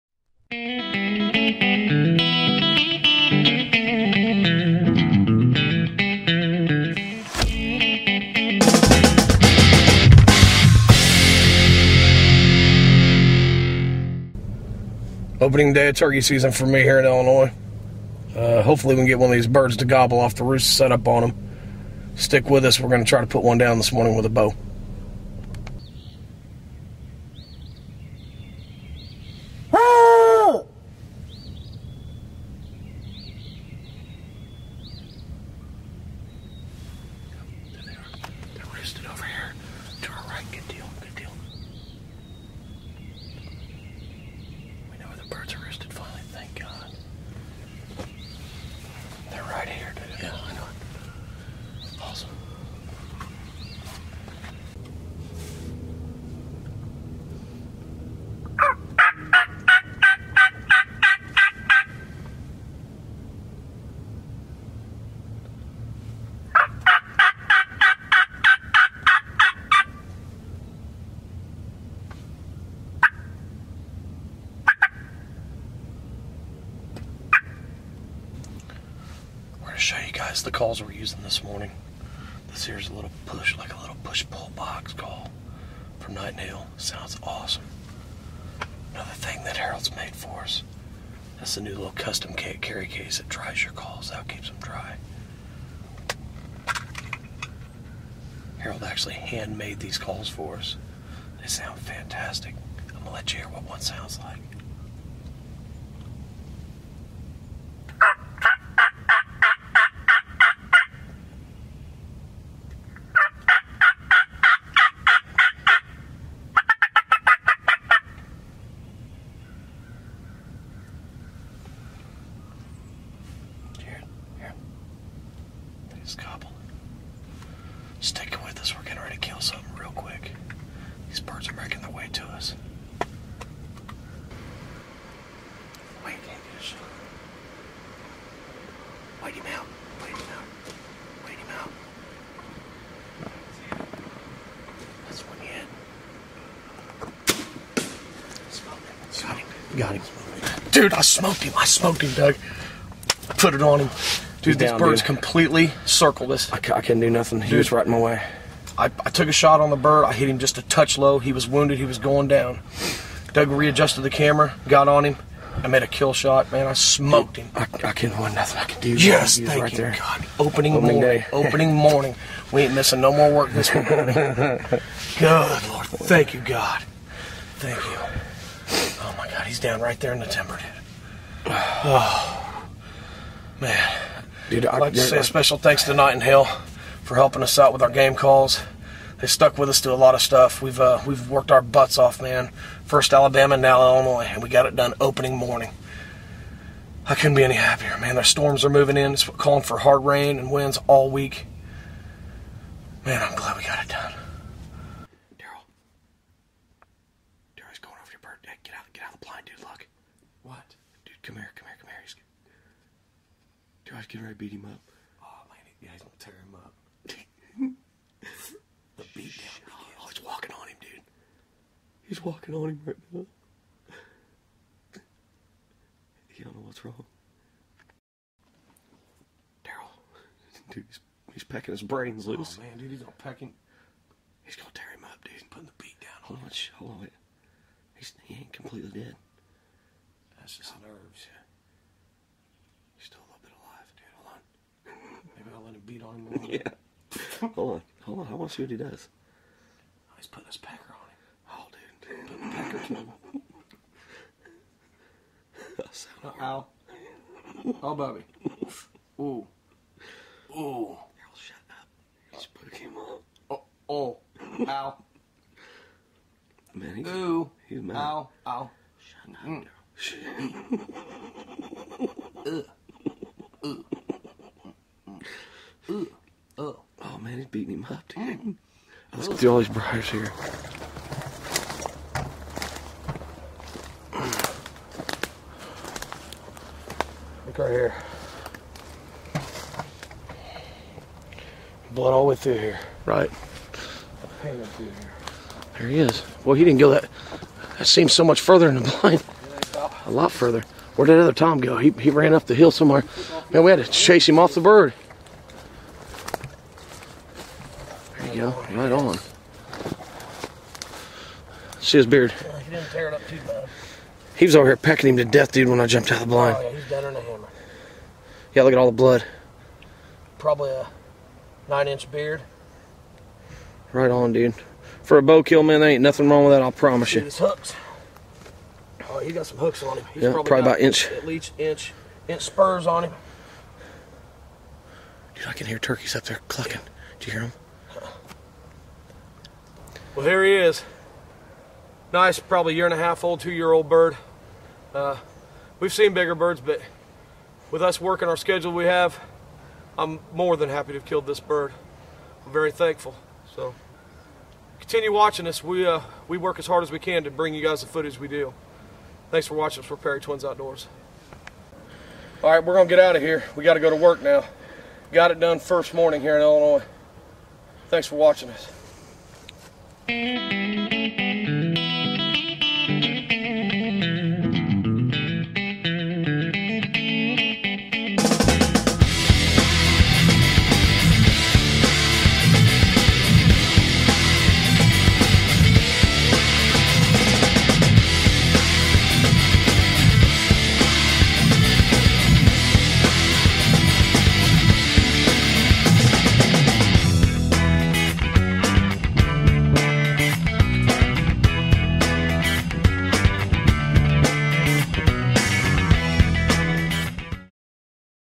bowhunting turkey kill 2013 perrytwins outdoors illinois
darrell perry kills a nice turkey opening day of illinois turkey season with his mathews bow, thanks to knight and hale game calls and millrich outdoors